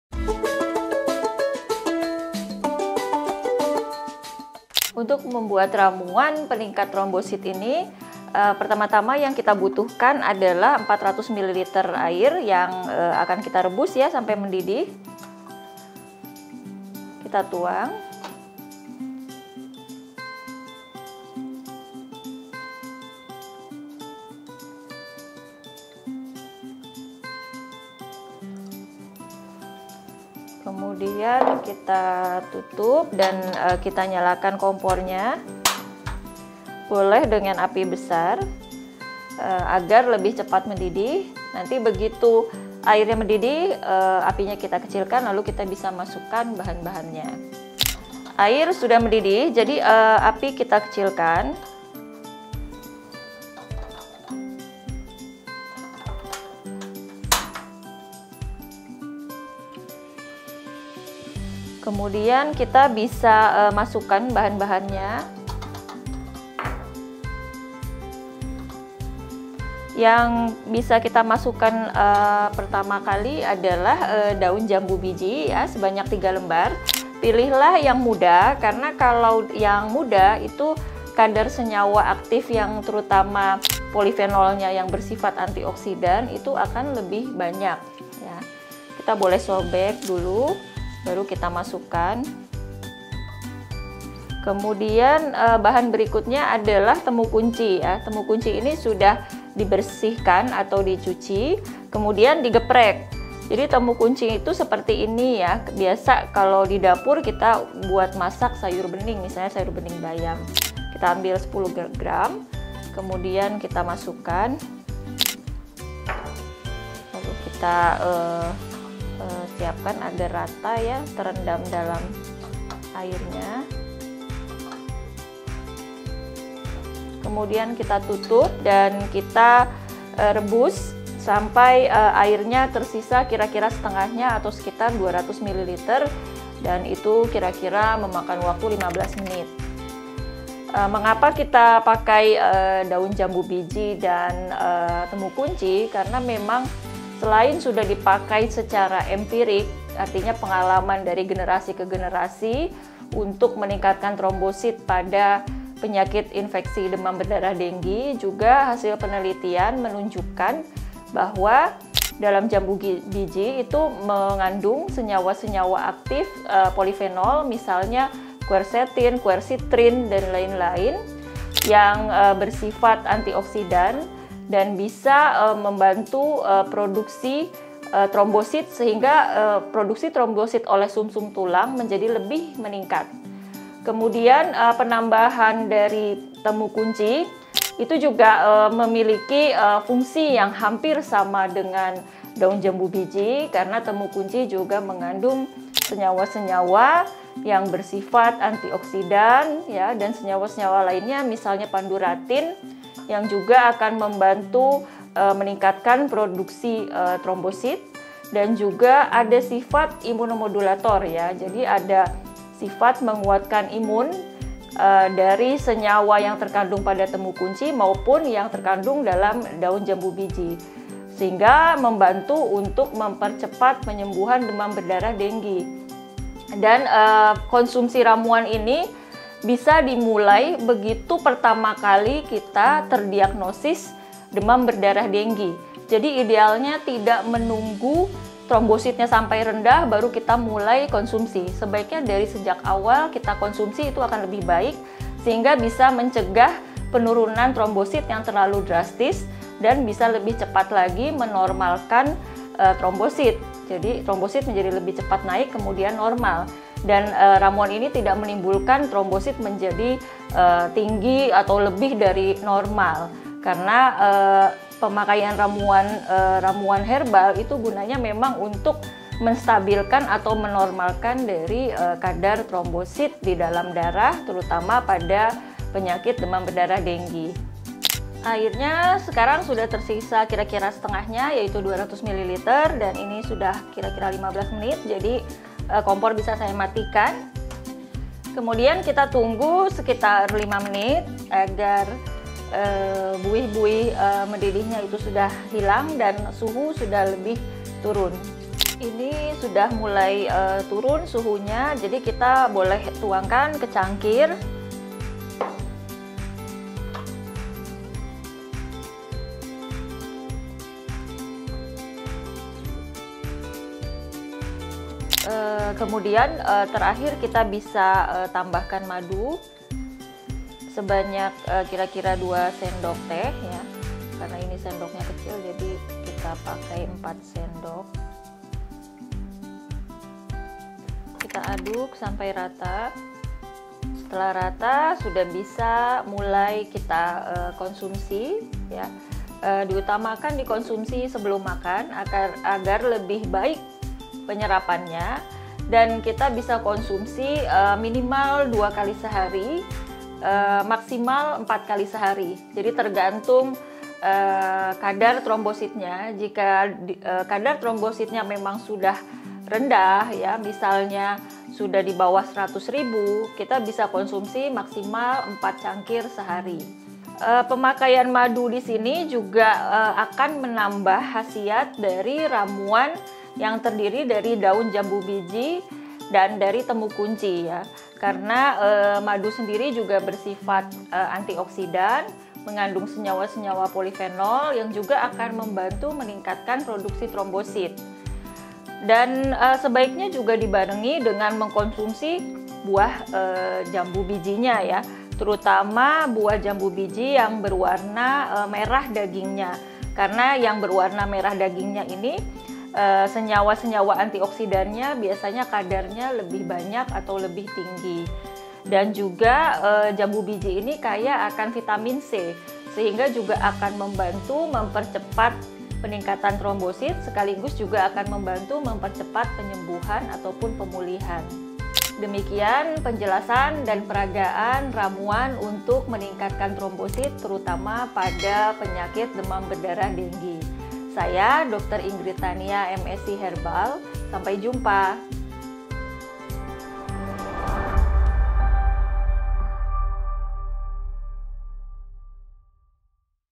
Meningkatkan Trombosit Saat DBD Secara Alami | Kata Dokter
VDVC health - Cara menaikkan trombosit secara alami ini sangat penting dilakukan bagi Anda yang sedang mengalami demam berdarah. Simak tips membuat ramuan alami untuk meningkatkan trombosit darah, bersama DR. (Cand.) dr. Inggrid Tania, M.Si . (Herbal). Yuk, simak dalam episode Nutrisi & Herbal berikut ini.